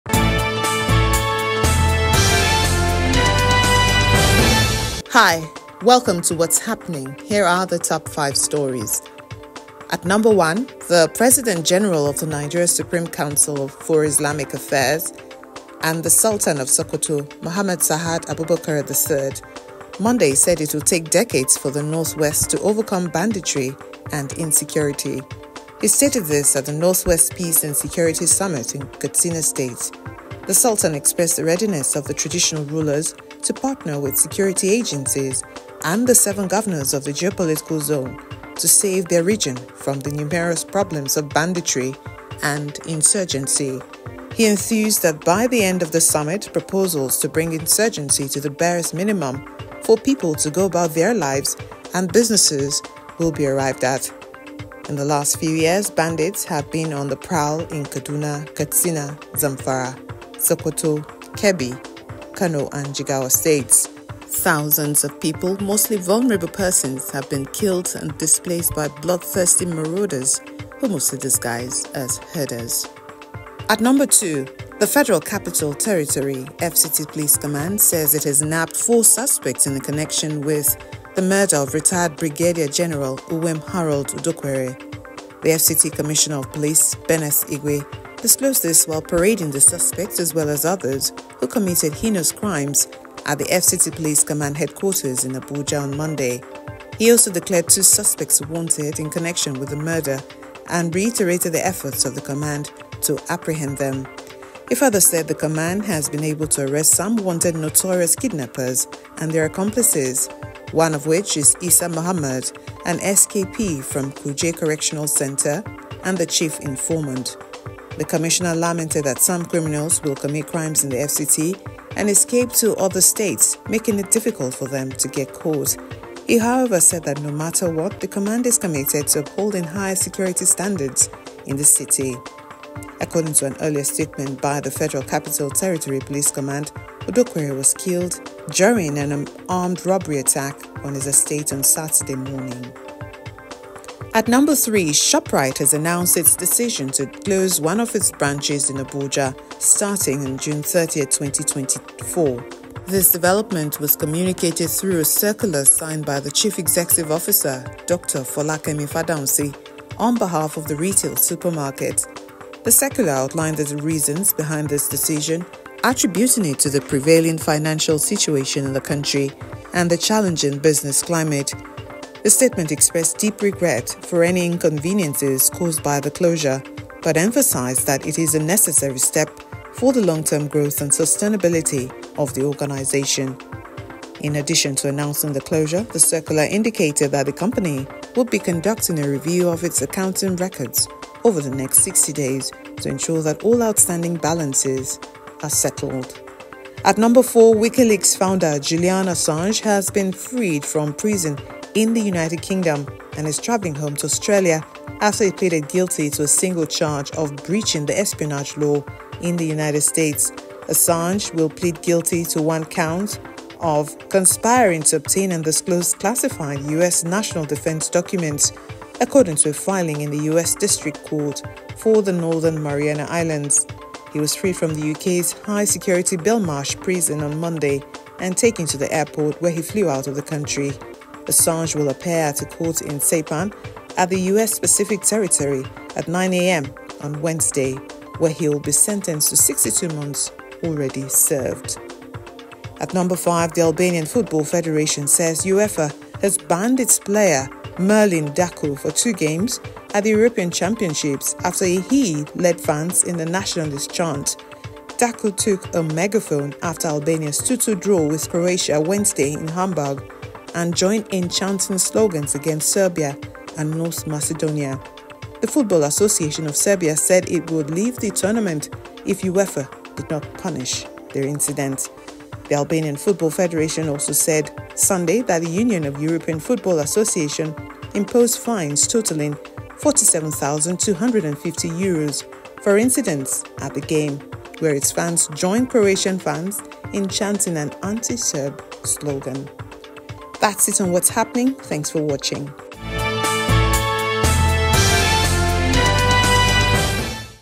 TOP 5 NEWS
1. It Will Take Decades To Get Out Of Banditry – Sultan
2. Police Arrest Four Suspects Over Murder Of Retired General In Abuja
3. Shoprite to shut down Abuja branch June 30
4. Wikileaks founder Julian Assange released from prison after US plea deal
5. Albania Player Banned For Two Euro 2024 Games For ‘Nationalist’ Chants

Follow Rootstv Nigeria's latest headlines on politics, sports, and entertainment